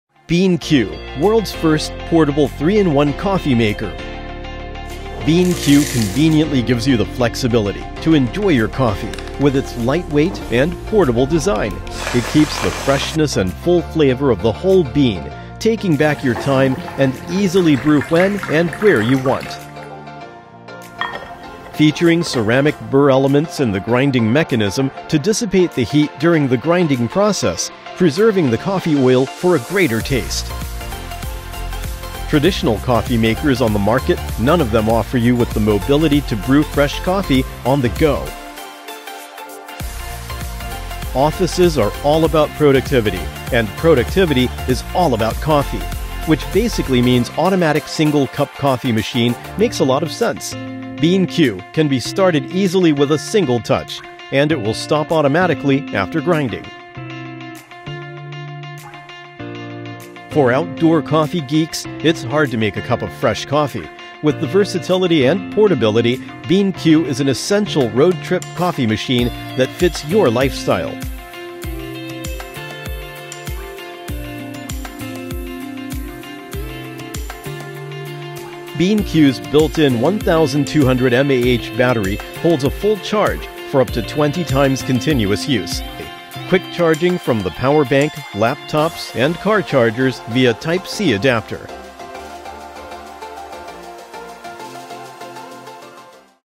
Beanque, 3-In-1 On-the-go Automatic Coffee Maker
Rechargeable 3-in-1 coffee maker makes fresh ground coffee anywhere with one-touch auto-grind & brew & travel mug.

Nowadays, coffee makers can do far more than just make a pot of Joe. Normal coffee makers on the market are expensive to purchase and time-consuming for operating, especially only for indoor use due to their bulky-size.

There are two types of coffee drinkers: people who drink coffee and people who are obsessed with coffee. Most of us who love coffee have brewing equipment in our kitchen, but when we travel or go camping, we often endure whatever is available – sometimes even instant coffee. Over the past decade, advances in coffee brewing technology, availability and affordability have made it relatively easy to make a cup of fine coffee anywhere. But for coffee geeks, there're still gaps between the perfect fresh coffee in their minds. Now, Beanque is exactly born for this.

Introducing Beanque, a portable 3-in-1 grind & brew & travel cup coffee maker. The built-in electric grinder allows you to convert the beans into freshly ground coffee. It can be started easily with a single touch, and it will stop automatically after grinding. Brew a cup of fresher coffee and store it in its handheld mug. Beanque is your ideal partner for making superior coffee on the go.

For outdoor coffee geeks, it's hard to make a cup of fresh coffee. With the versatility and portability, Beanque is an essential road trip coffee machine that fits your lifestyle.

Many coffee drinkers prefer grinding fresh beans right before brewing. It can maintain the flavor of the coffee and enhance the quality. How many separated accessories are required to make a cup of perfect coffee? Grinder, filter, coffee beans, measuring cups, etc..

Don’t worry! Beanque fits all you need. It stores up to 50 grams of coffee beans at a time and comes standard with a measuring cup and filters, which eliminates the need to measure your coffee beans with a spoon and makes it great for portability.

Brewing your coffee after grinding that can keep it nice and hot in a self-contained S/S vacuum travel mug. This is a great feature for those who need a portable coffee maker for camping or hiking.

Coffee lovers hold many opinions regarding the ideal water-to-coffee ratio, which can differ from the grind of the beans. Beanque comes with a multi-grade adjustment knob that you can infinitely adjust the grinding fineness to your coffee preference, and effortlessly make perfect ground coffee you’d expect.

Quick charging from the power bank, laptops and car chargers via Type-C adapter. Beanque’s built-in 1200mAh battery holds a full charge for up to 10 times continuous use, which makes it a great solution for camping since you won’t have to charge it constantly.